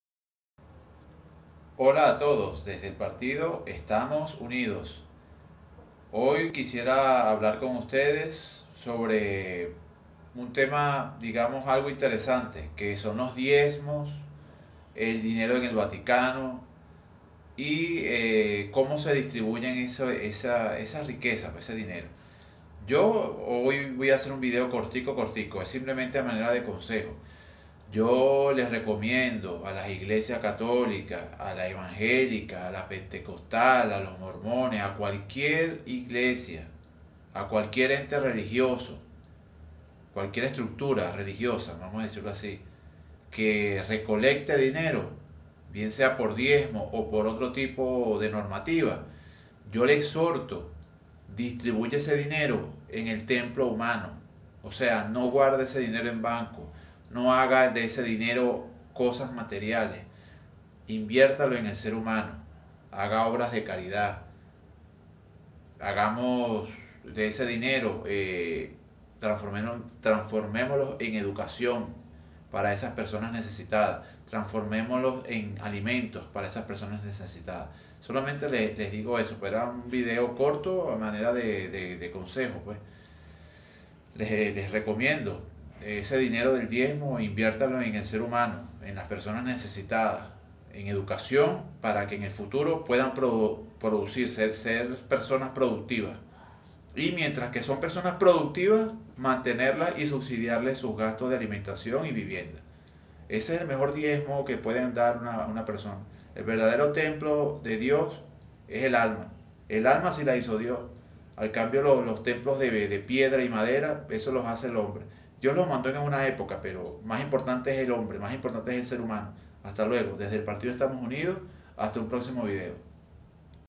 LOS DIEZMOS Y LAS RIQUEZAS DEL VATICANO,DONENLOS.
EL VERDADERO DIEZMO DEBEN DE DARSELO A LOS POBRES Y PERSONAS NECESITADAS DE TODO EL MUNDO